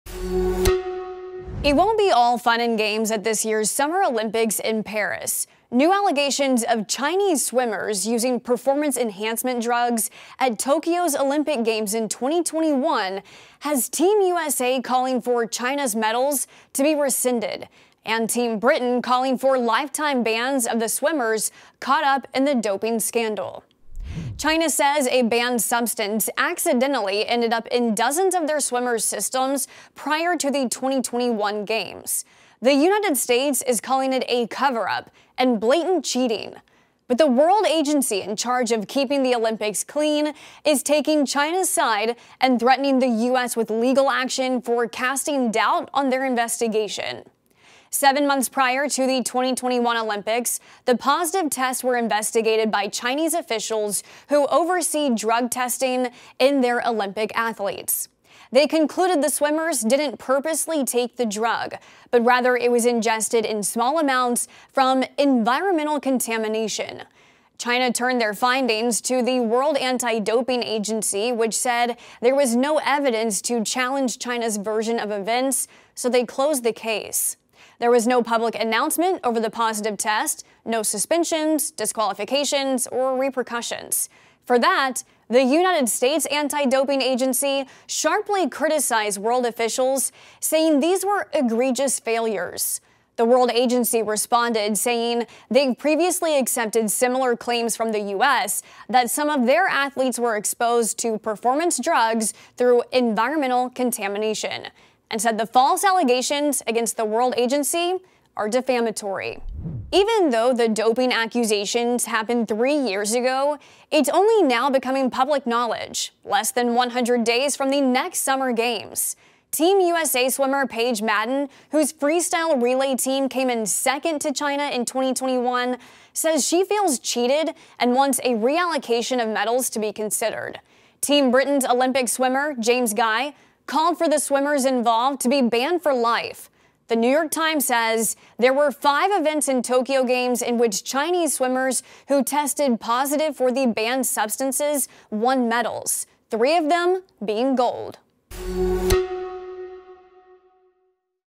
Chinese doping scandal hits 3 months before Paris Games; US claims cover-up
As the 2024 Paris Summer Olympics approach, new allegations of Chinese swimmers using performance enhancement drugs at the 2021 Tokyo Olympics are appearing. Team USA is calling for China’s medals to be rescinded and Team Britain is calling for a lifetime ban of the swimmers involved in the doping scandal.